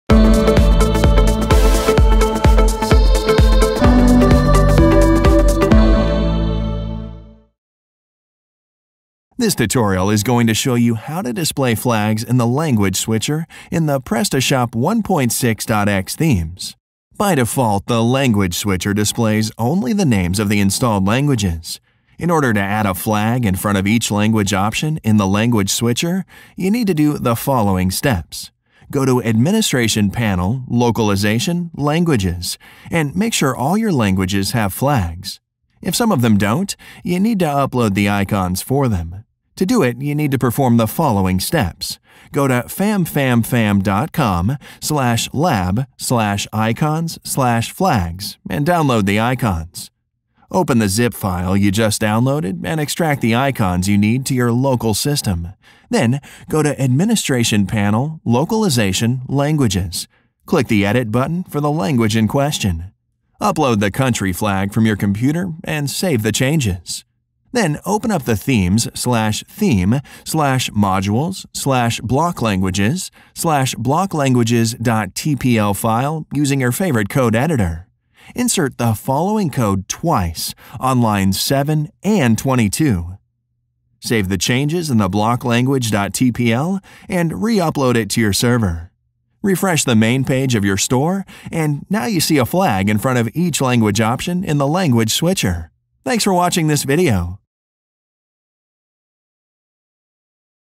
PrestaShop 1.6.x. How To Display Flags In The Language Switcher
This tutorial is going to show you how to display flags in the language switcher in your PrestaShop template.